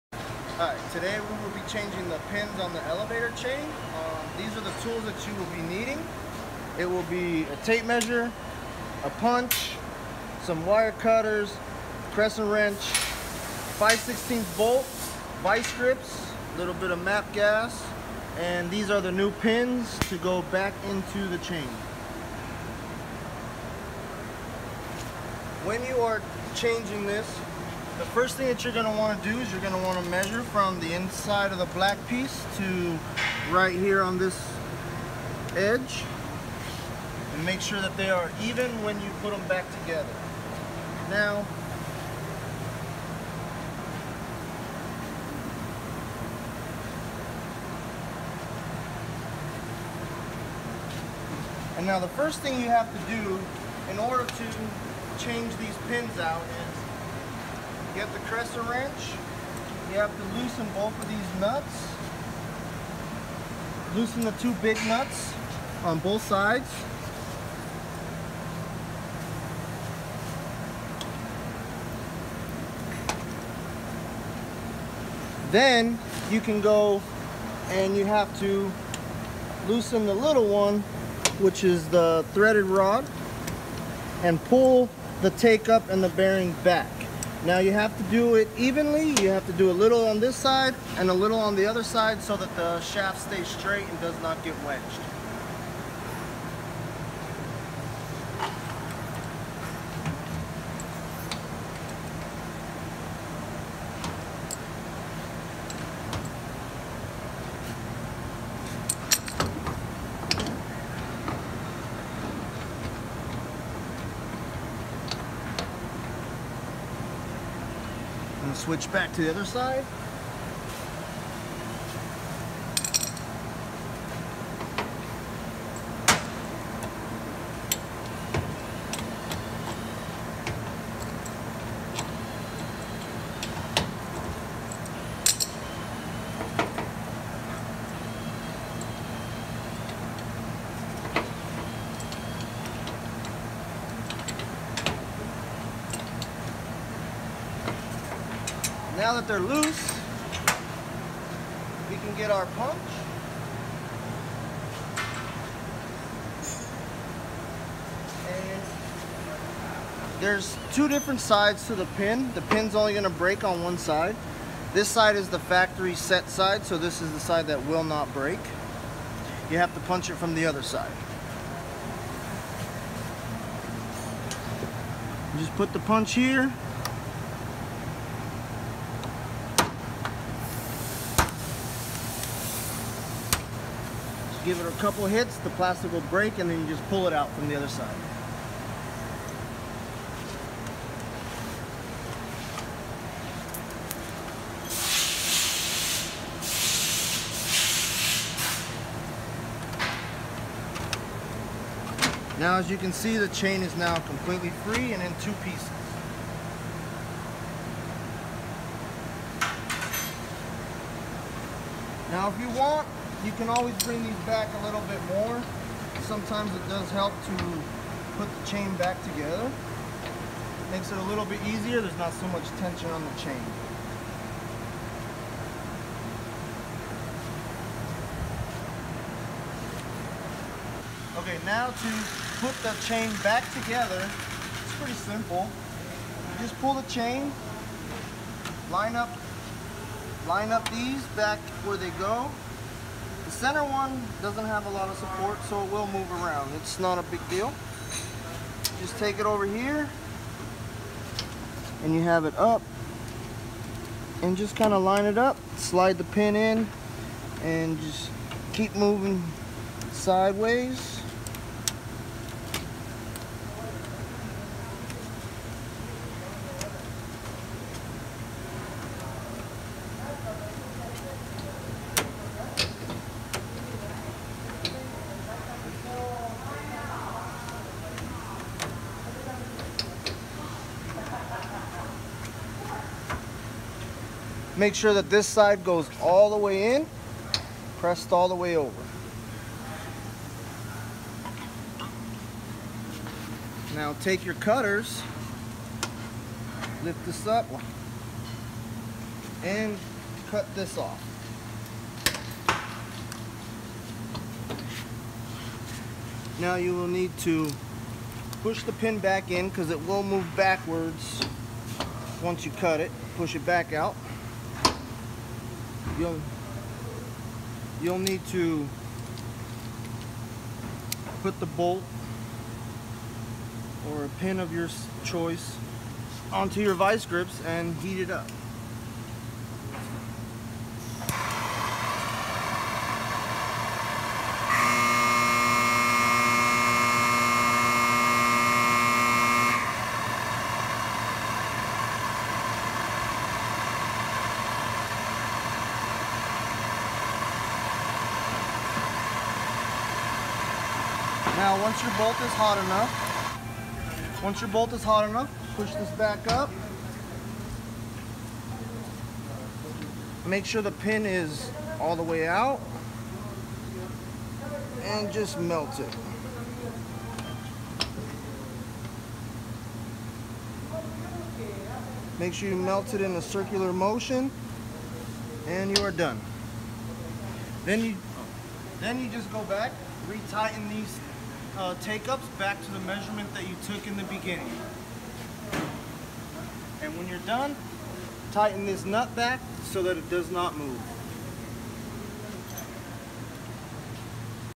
Resina HES Elevator Pin Replacement
Instructional Video on how to replace or add flight sections using pin replacement